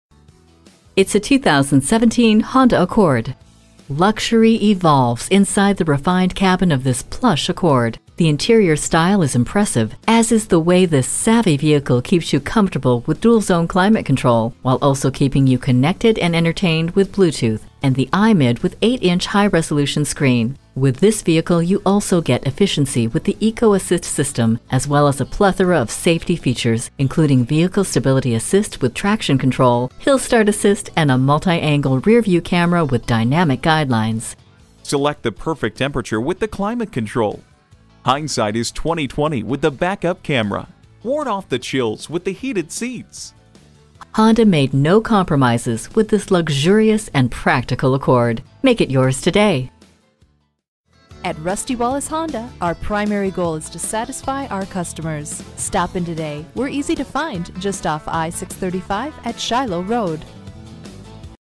2017 Honda Accord Sedan Dallas TX Fort Worth, TX #170421 - SOLD
Year: 2017
Make: Honda
Model: Accord Sedan
Trim: EX-L CVT
Engine: 4 Cyl - 2.4 L
Transmission: Variable
Color: Nh-731px/Crystal B
Interior: Nh-731px/Ivory
Stock #: 170421
VIN: 1HGCR2F81HA094263

Address:
12277 Shiloh Road